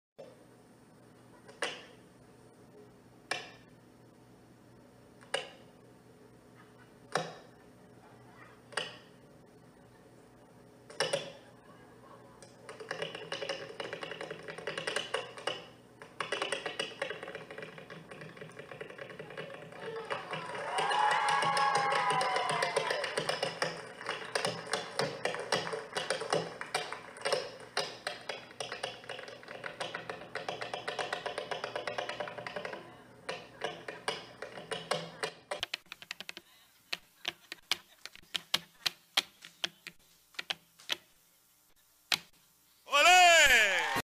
William Diaz es el bailarín JOAQUIN CORTES
El imitador y humorista dominicano personifica al famosísimo bailarín de flamenco el español Joaquín  Cortes. Síguenos por Instagram @willimsdiaz